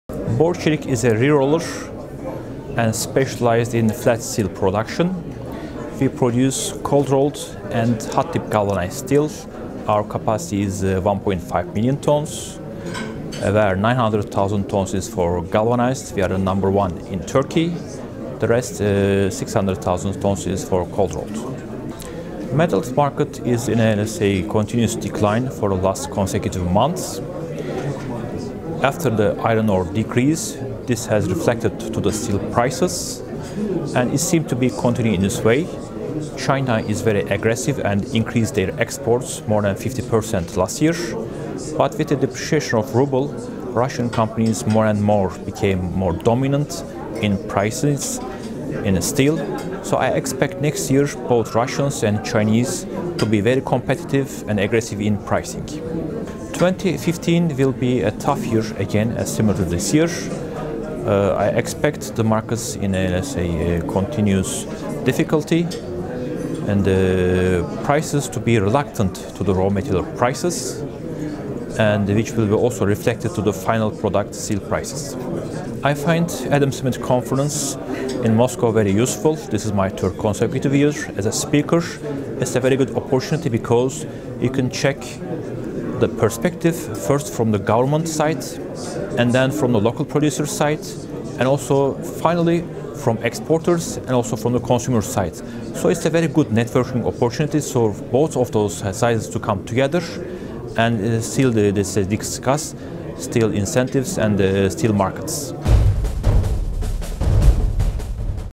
Ersun Ozdemirel, Borcelik at Russian & CIS Metals Summit 2015
Interview with Ersun Ozdemirel, Exports Director of Borcelik at the 20th Russian & CIS Metals Summit, as part of the Russian & CIS Metals and Mining week which took place at the InterContinental Moscow Tverskaya Hotel.

Adam Smith Conferences